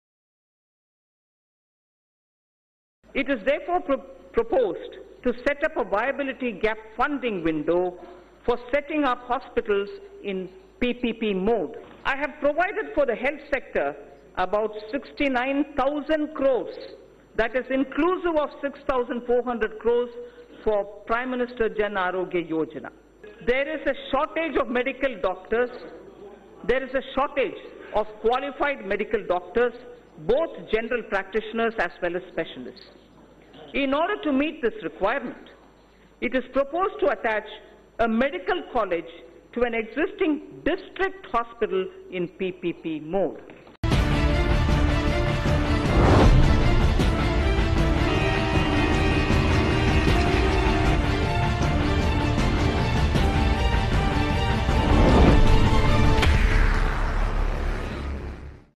২0,000 কিলোমিটার রেলপথের বিদ্যুতায়ন: নির্মলা সীতারমন । ABP Ananda
Electrification of 20,000 km railway: Nirmala Sitharaman.